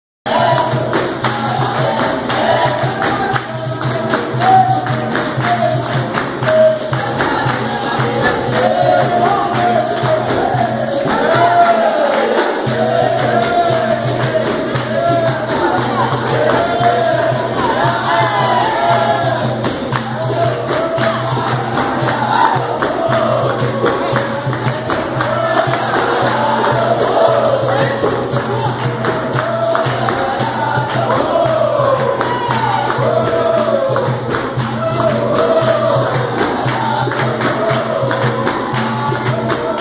Última Roda do ano na academia em 14/12/2014.
Familia Filhos de Aberrê
Roda de Confraternização..muito obrigado a todos que compareceram...AXÉ !!